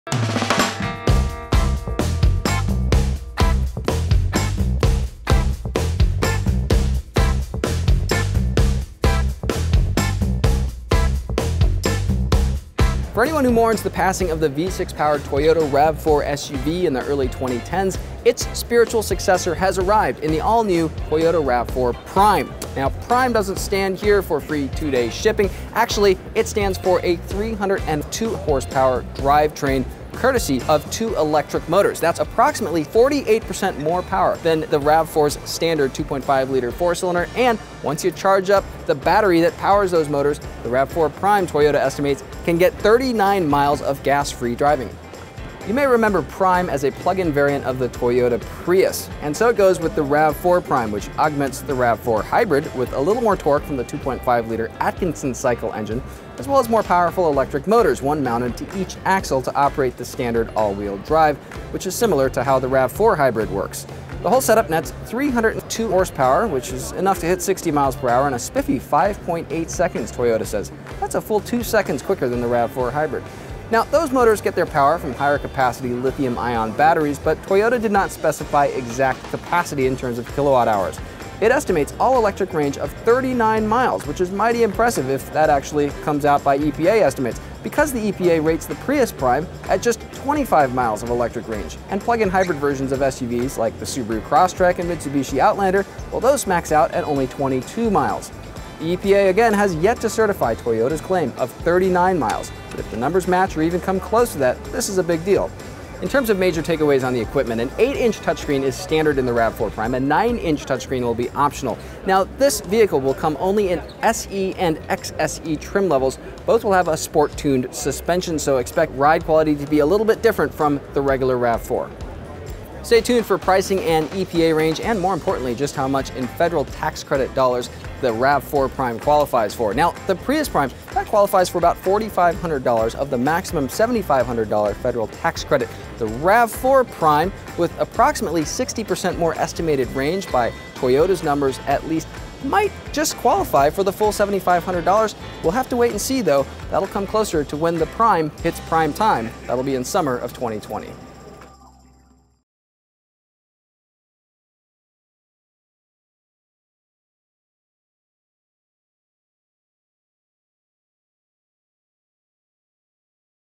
2021 Toyota RAV4 Prime: First Impressions – Cars.com
At the 2019 Los Angeles Auto Show, Toyota introduces the green and performance 2021Toyota RAV4 Prime, with 302 total horsepower — nearly 50% more than the gasoline models. Watch the video for more.